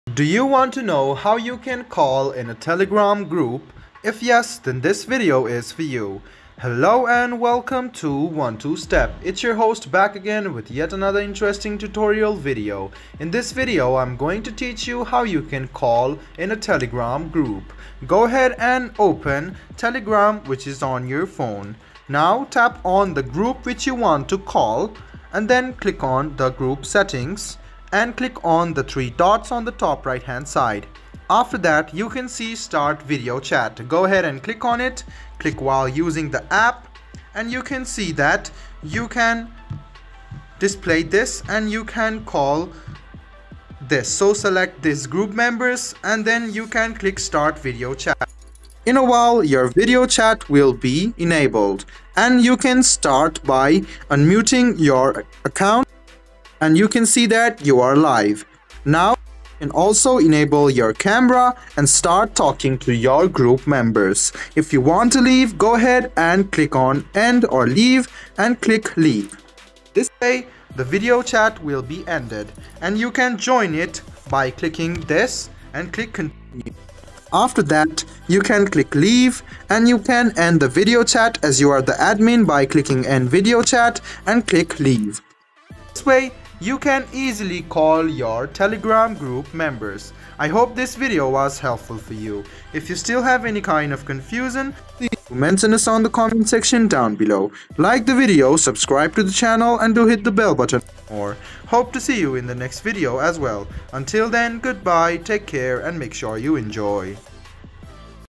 How to Make A Call on Telegram Group (2024)
Ever wished you could have a group conversation on Telegram with voices instead of just texts? Look no further! This video unveils the secrets of making calls within your Telegram groups. Learn how to initiate voice chats, invite specific members, and even switch to video calls for a more face-to-face experience.